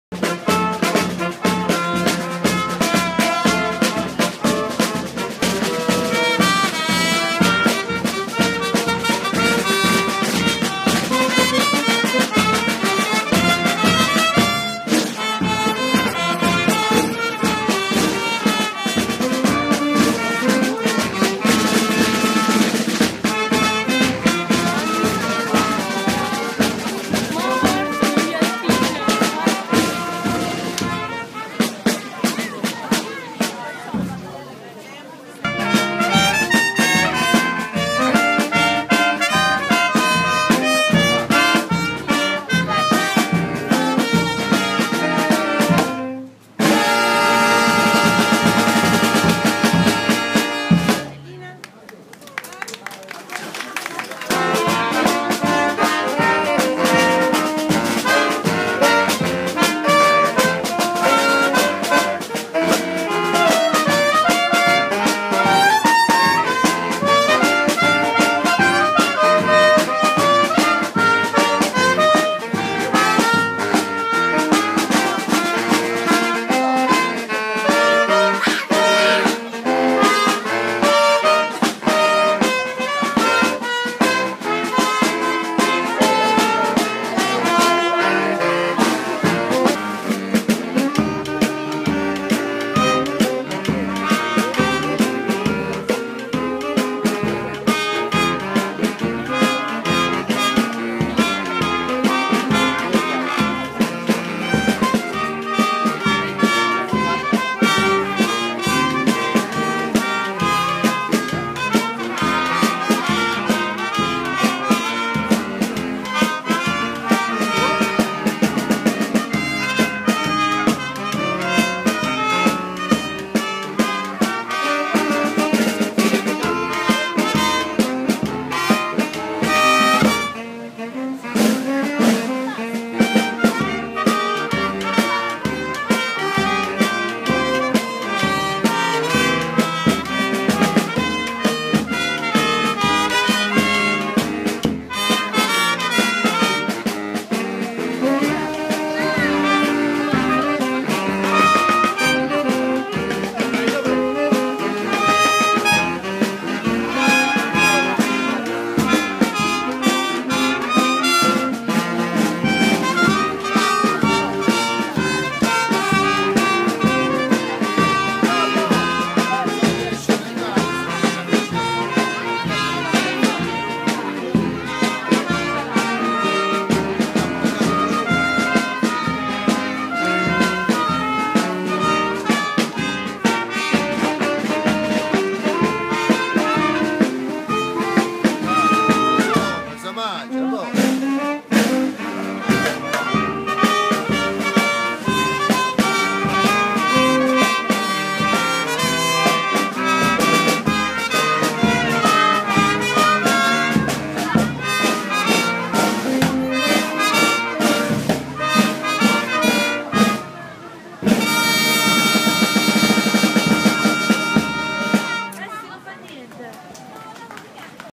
Agnone Cilento dicembre.
Ad Agnone-Cilento è già un senso di festa. C'è una settimana prima di Natale. E gli abitanti di questa piccola città del Sud Italia si stanno già divertendo e festeggiando.